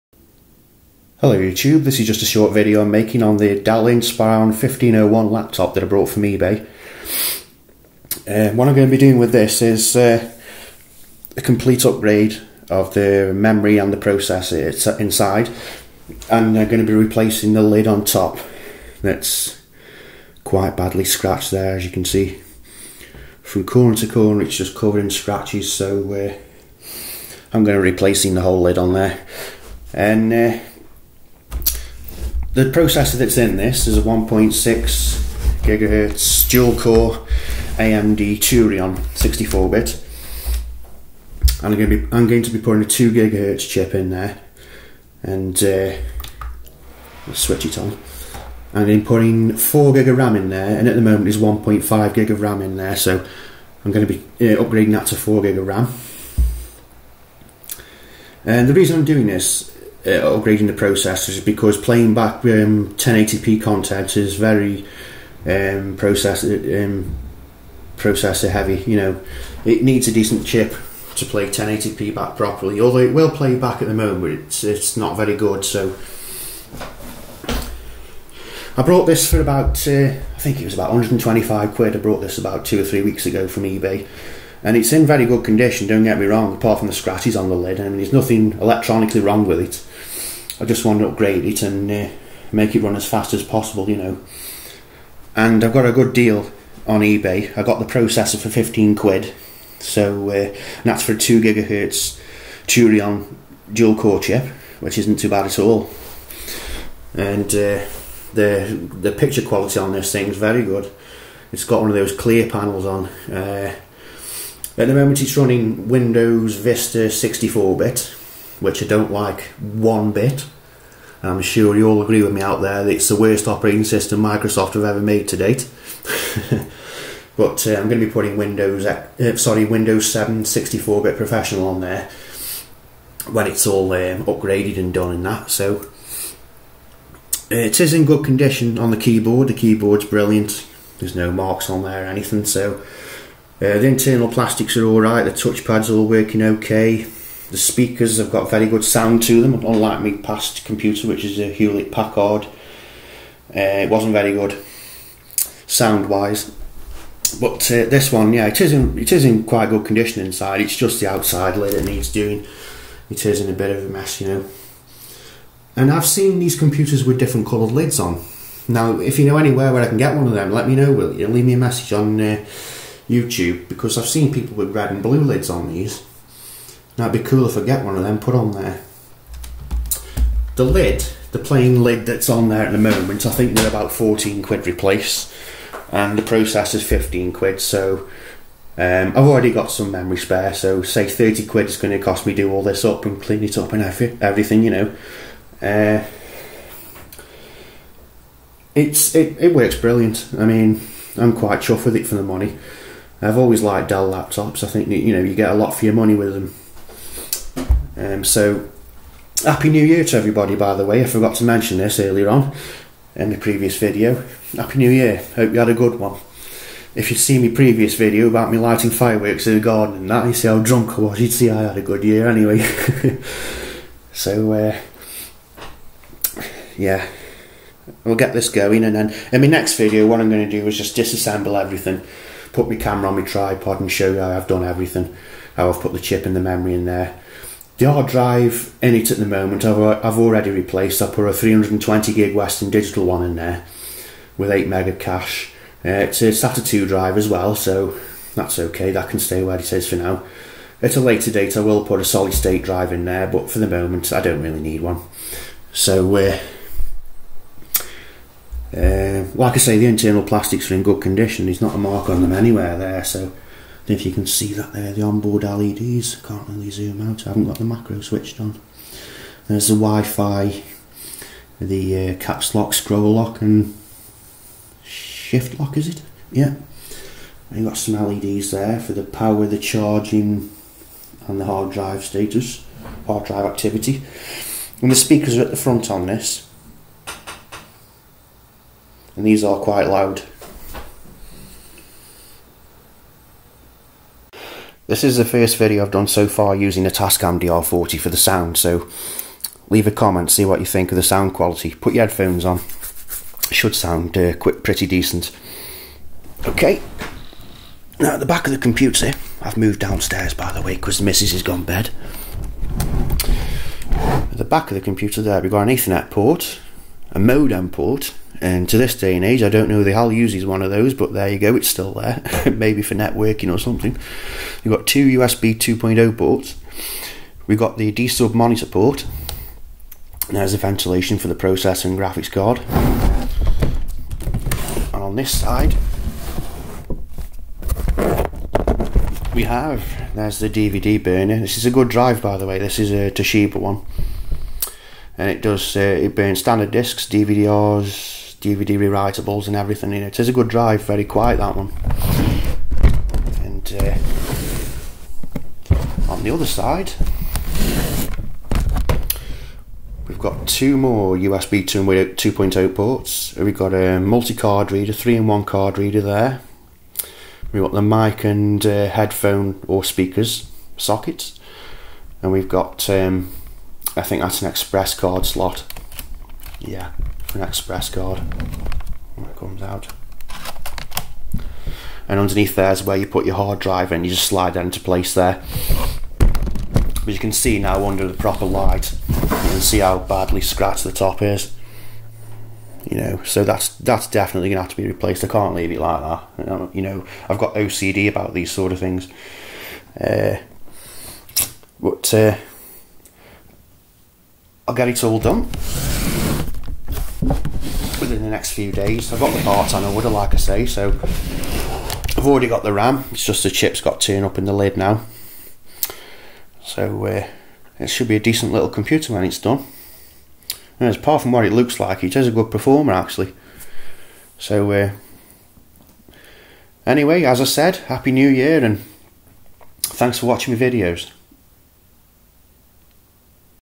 Dell Inspiron 1501 Upgrade & Repair Video 1 Of 3
Re edited with cyberlink power director, sound is now clear as a bell.

First of three videos of me upgrading and repairing the DELL inspiron 1501 laptop, bought from ebay for £125 it's a good performer, ive already replaced the hard drive from the standard 120GB toshiba to a 320GB western digital black 320GB SataII with 16MB cache. The CPU i will be installing will be an AMD Turion 64 X2 2.0GHz dual core, then 1080p playback will be good. These processors are the equivilents to intels core 2 duo chips, These are great laptops for the price i highly recommend them.

The graphics processor in this is an ATI radeon x1150